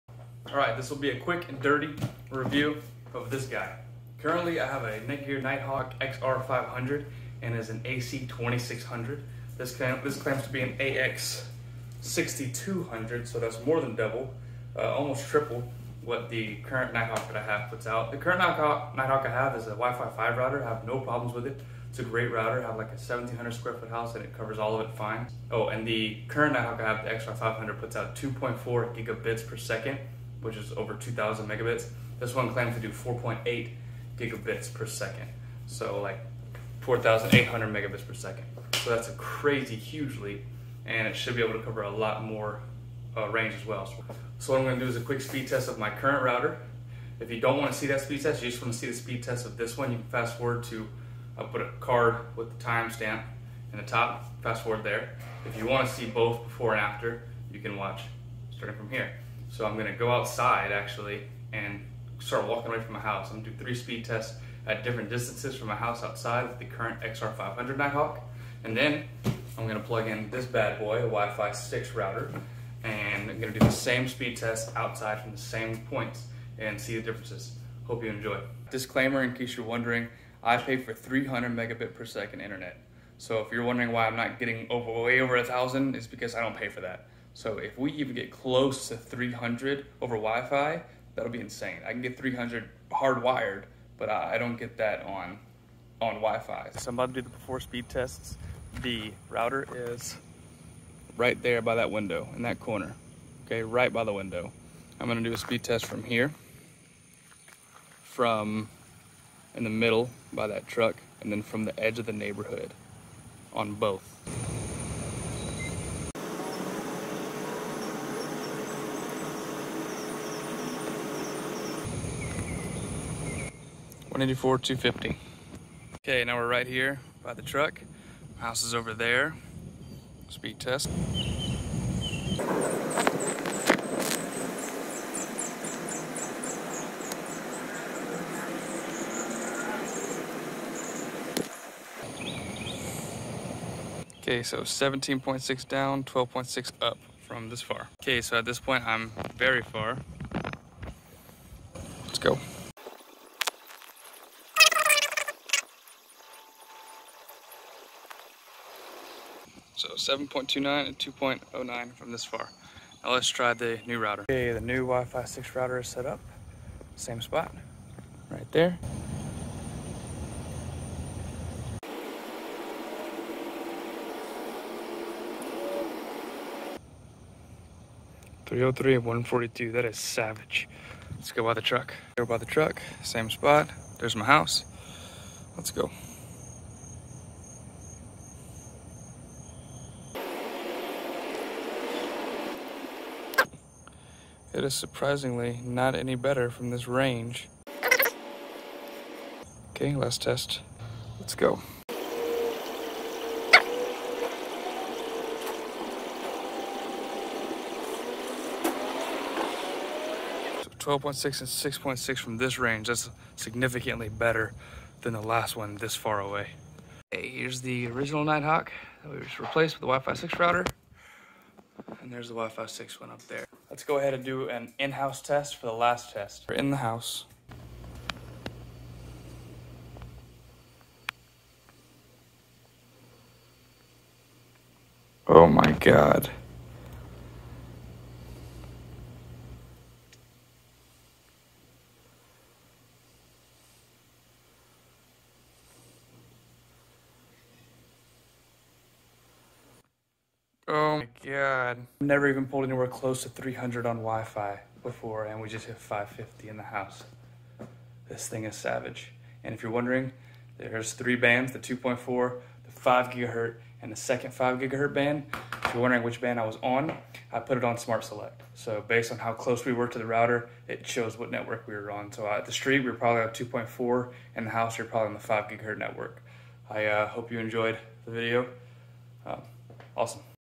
Nighthawk AX8 Wifi 6 Router vs Nighthawk XR500 Wifi5 Speed/Range Test.
I recently upgraded from a wifi 5 XR500 nighthawk to the wifi 6 AX8 nighthawk and did some quick speed tests of both from multiple ranges. If you were thinking of upgrading, or purchasing this router, I hope I helped you decide, thank you.